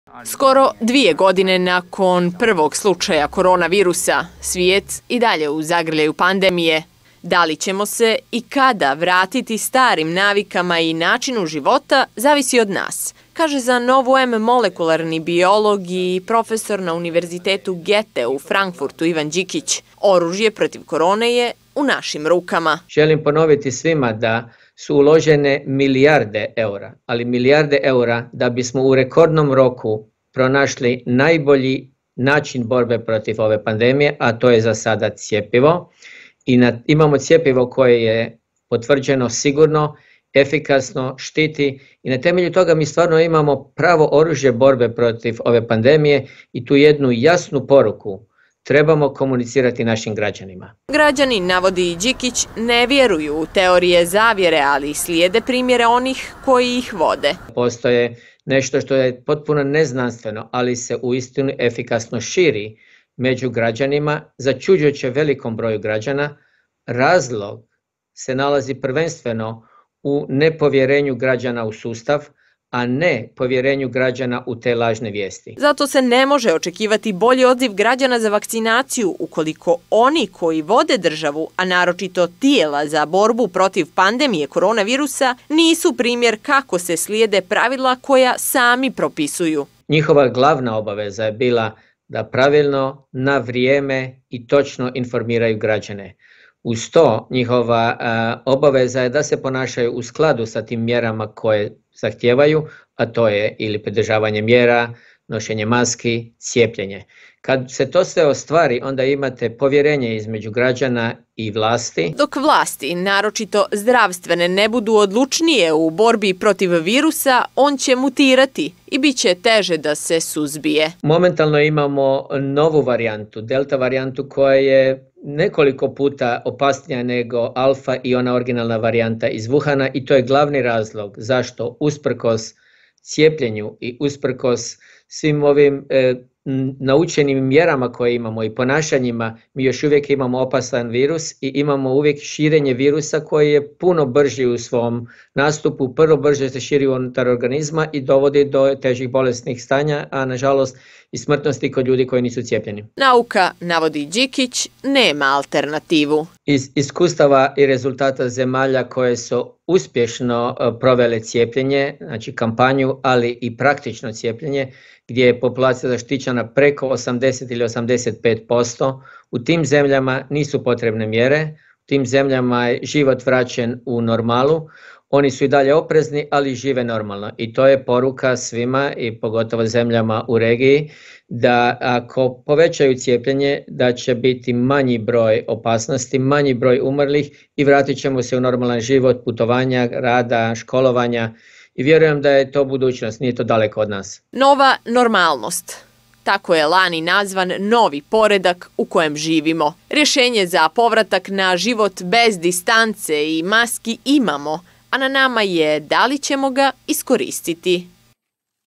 IVAN ĐIKIĆ MOLEKULARNI BIOLOG ZA NOVU M, IVANA POPOVIĆ 17. OKTOBAR 2021
Nazire li se kraj pandemiji koronavirusa? U većini zemalja na snazi su epidemiološke mjere. Svjetska zdravstvena organizacija odobrila je više vakcina, imunizacija se vrši širom svijeta, a zdravstvene vlasti najavljuju nove talase epidemije. Zbog čega je to tako i kada ćemo se vratiti starim navikama, pitali smo naučnika sa Univerziteta Gete u Franfurtu, Ivana Đikića.